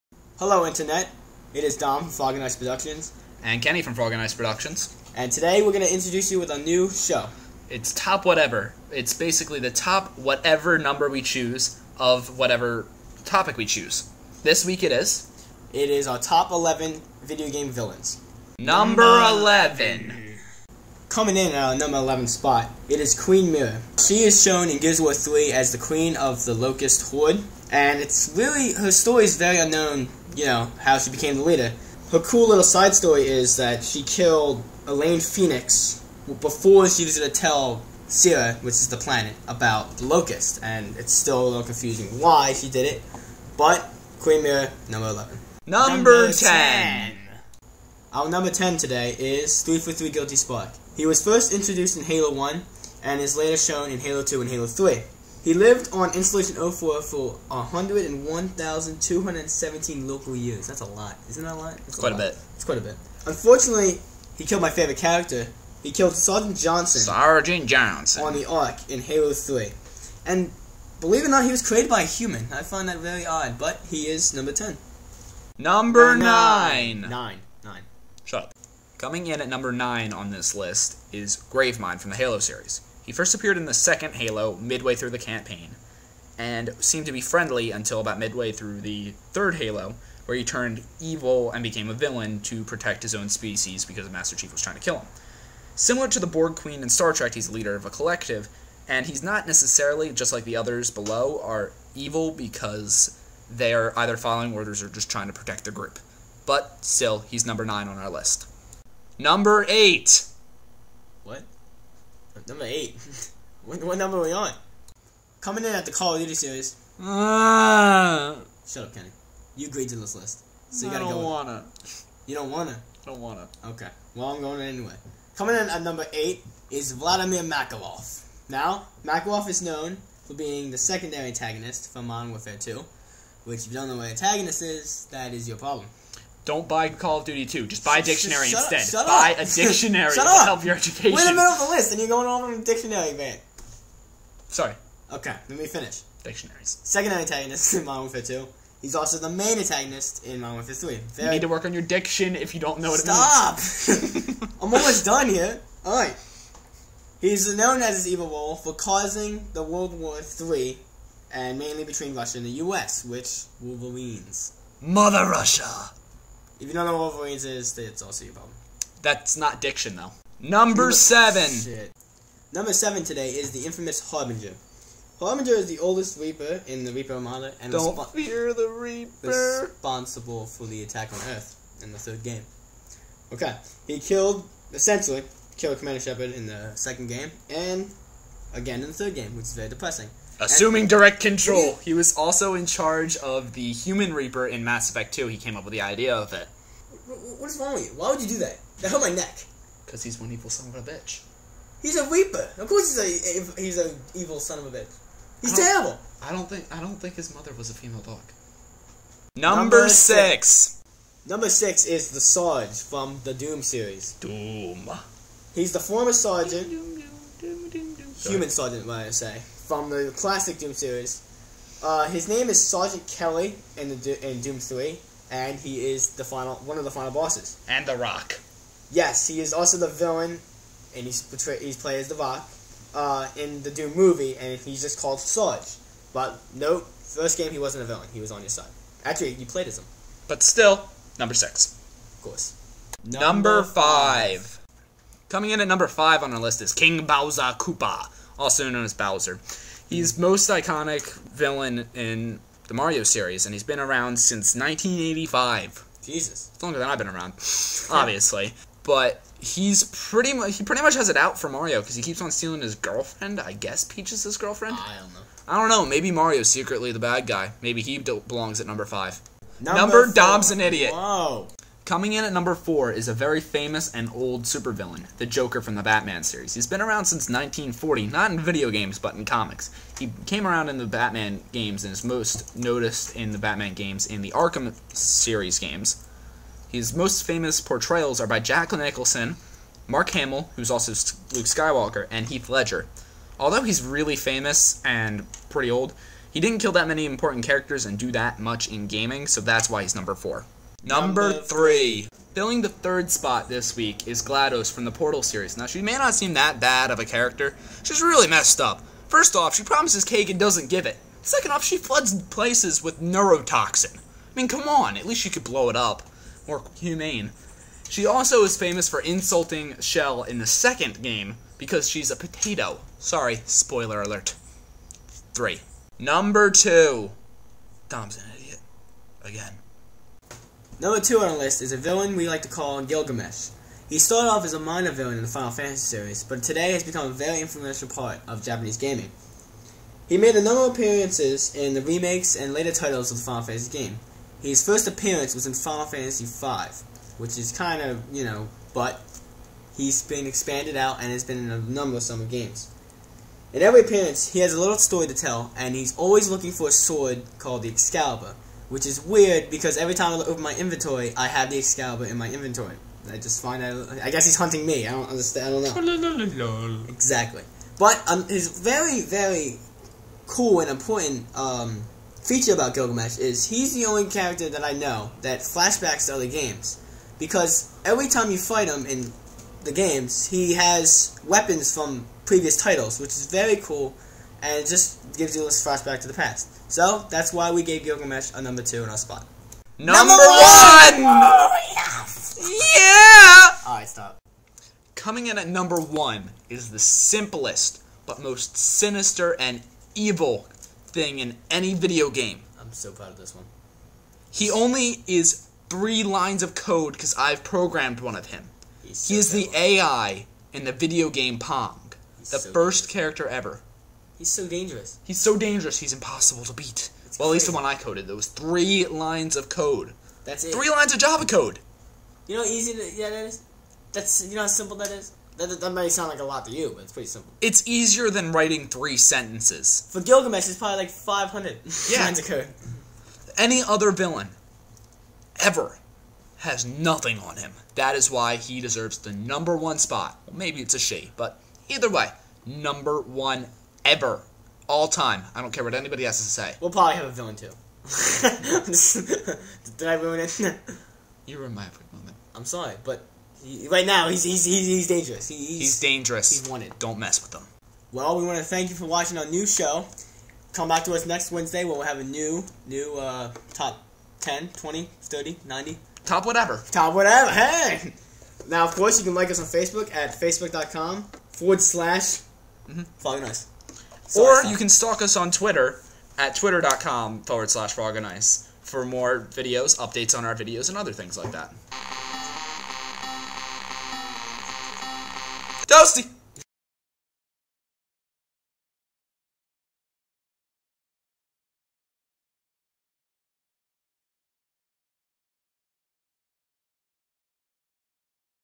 Top Whatever #1 Villains
Welcome to our New show, top whatever! This week, Top 11 Villains in Gaming!
Like our Stuff?